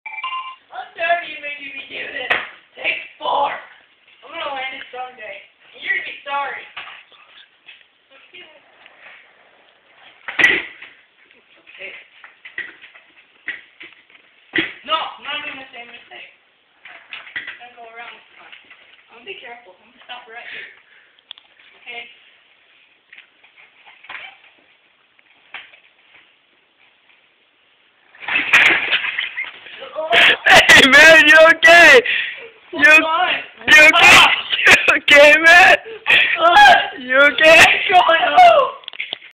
NEVER TRY THIS AT HOME (4)
MY FRIEND CANT SKATEBOARD TO SAVE HIS LIFE HAHAH URBAN JACKASS !